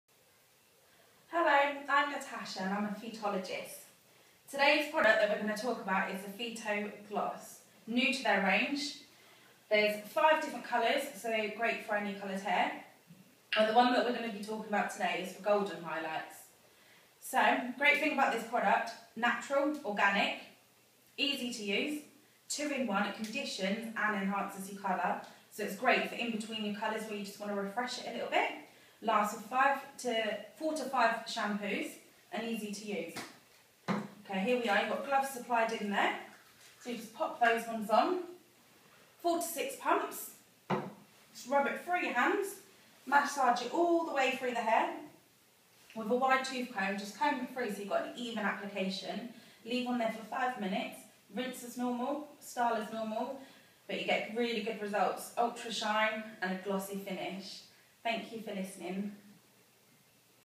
PHYTO PHYTOGLOSS GOLDEN , Highlights hair, unbelievable shine a gloss, no colour fade
Phyto PhytoGloss has two benefits in one application: Colours and Conditions.

- Express colour-enhancing treatment: highlights in only 5 minutes.
- Formula made from 95% natural ingredients.
- Exceptional shine.
- Long-lasting effect: 4 to 5 shampoos.
- Does not make hair become dry.
- Silicone and Paraben free.
- No oxidising agents, bleach-free or ammonia.